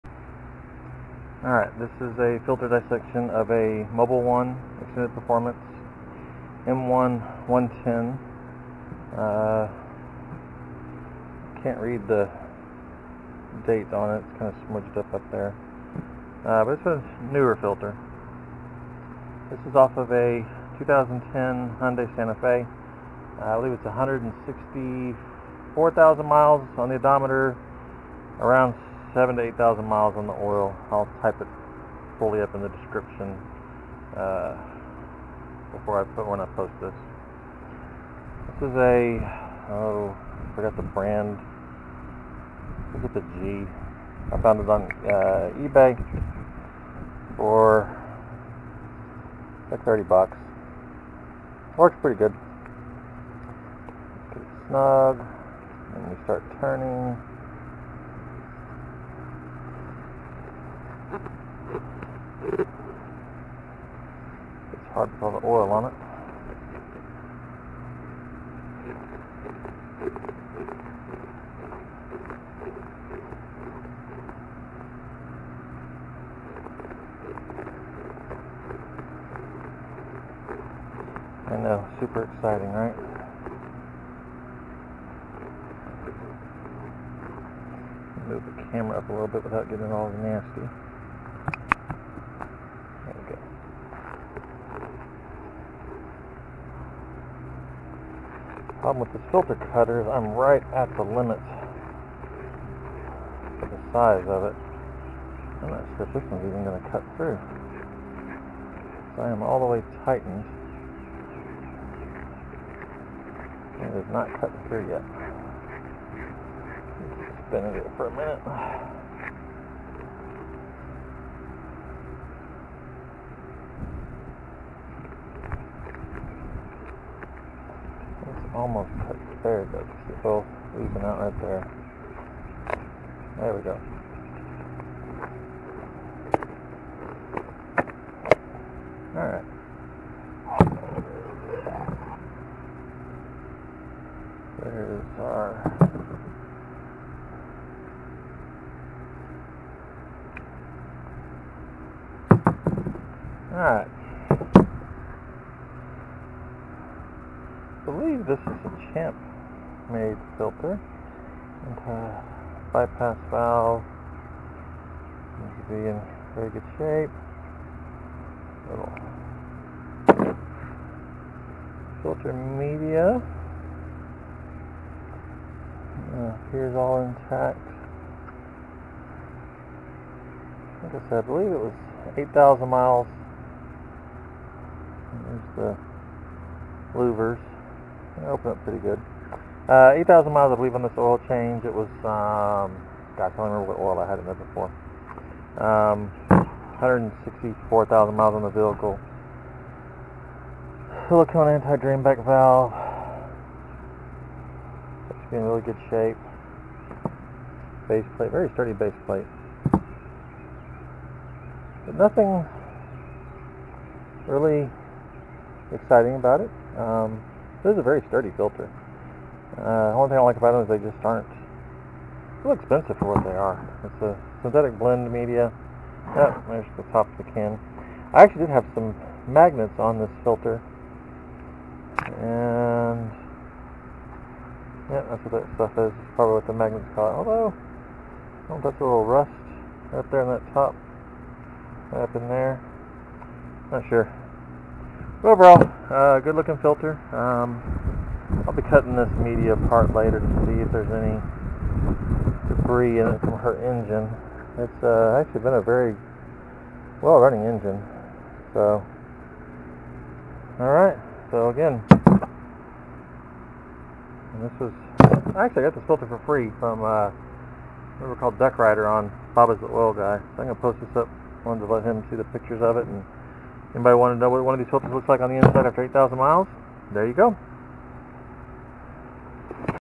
Mobil 1 M1-110 EP oil filter
This is a Mobil 1 M1-110 EP oil filter from a 2010 Hyundai Santa Fe.
7,823 miles/8 months on the filter, 162, 863 miles on vehicle.